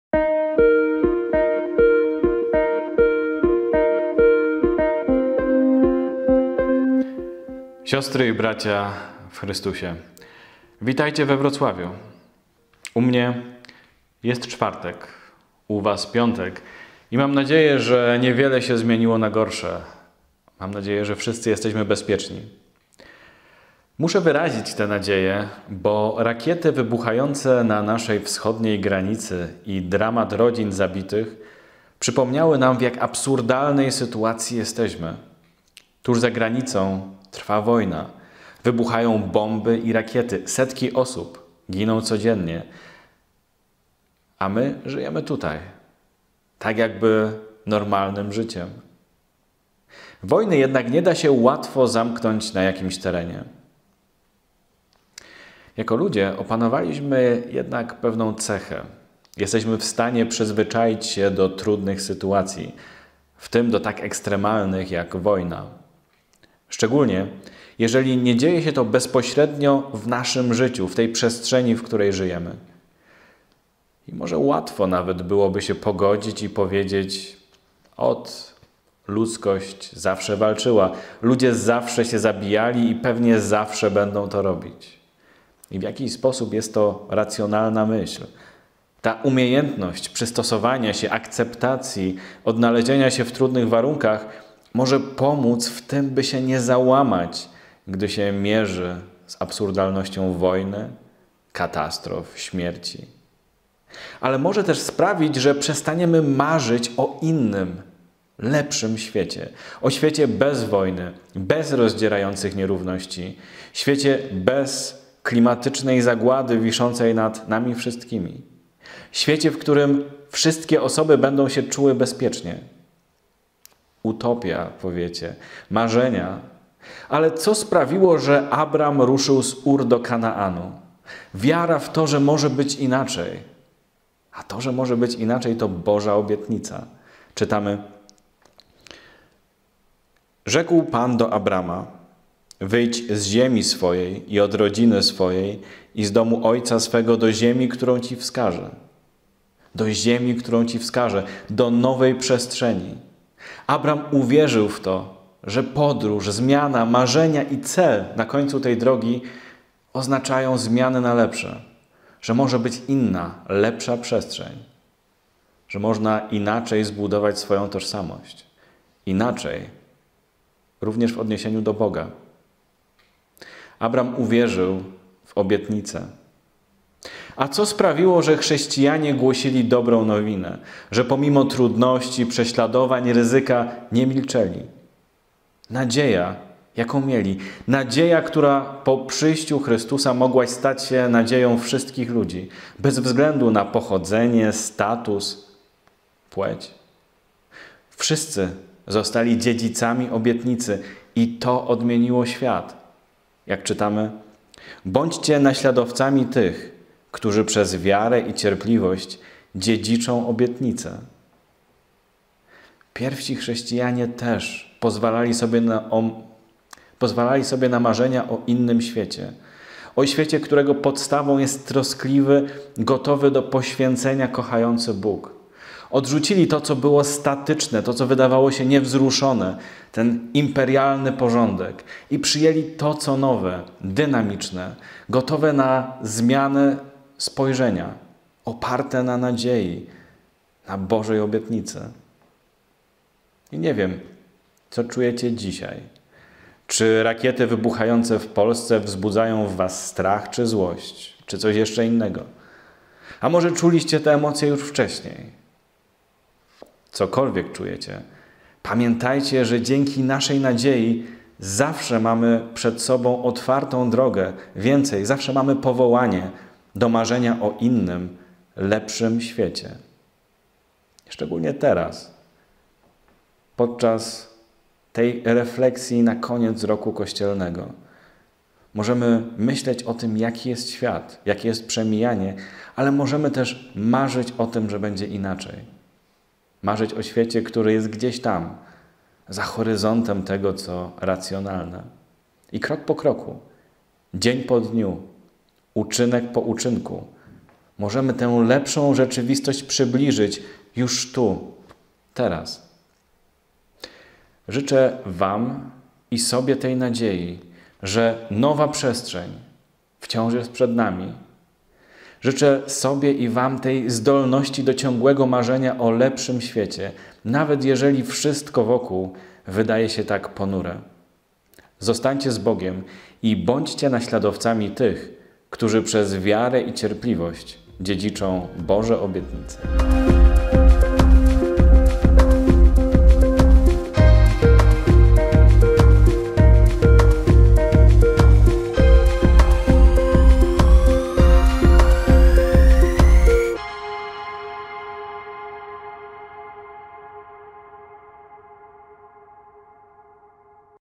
Codziennik biblijny, Słowo na dzień 18 listopada 2022 r.
ks. Paweł Mikołajczyk, Parafia Ewangelicko-Augsburska Opatrzności Bożej we Wrocławiu
Rozważanie biblijne na Słowo dnia:

Rzekł Pan do Abrama: Wyjdź z ziemi swojej i od rodziny swojej, i z domu ojca swego do ziemi, którą ci wskażę.
Pierwsza Księga Mojżeszowa 12,1

[Bądźcie] naśladowcami tych, którzy przez wiarę i cierpliwość dziedziczą obietnice.
List do Hebrajczyków 6,12
Codziennie o 7 rano spotkanie z Biblią.